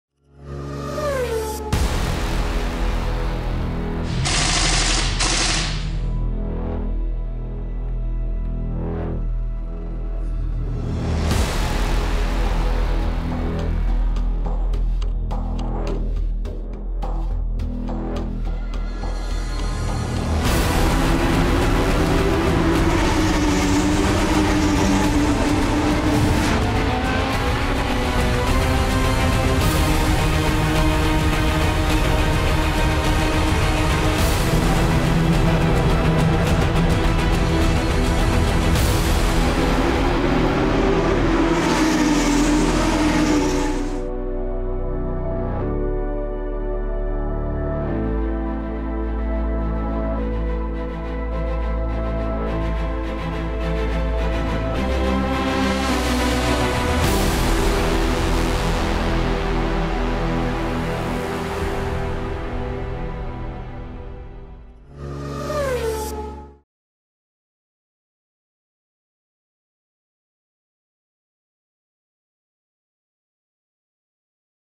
Highlights Lamborghini World Final 2018
Lo scorso fine settimana il circuito di Vallelunga è stato teatro dell'epica battaglia tra le Huracán Super Trofeo Evo, per decretare i campioni continentali dei Super Trofeo Europa, Nord America e Asia e i campioni del Mondo 2018.